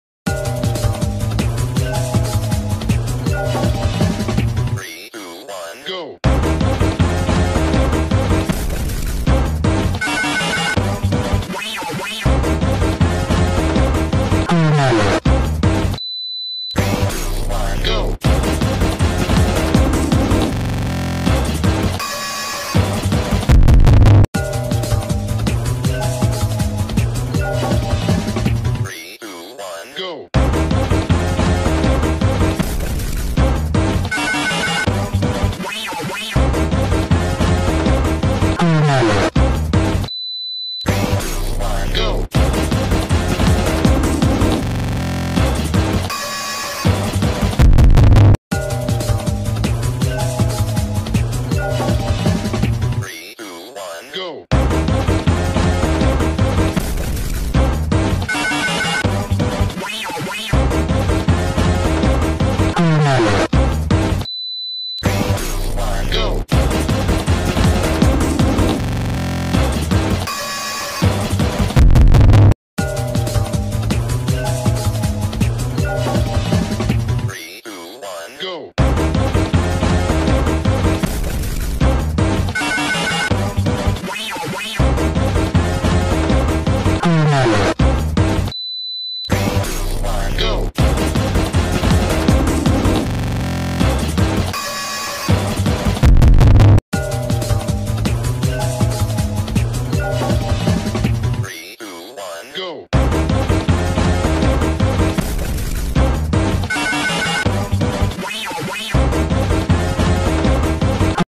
3..2..1... go!!!! F1
most f1 car crash goes to red bull .1
                                                 ferrari.2
                                                 alfa romeo .3

dangerous crash haas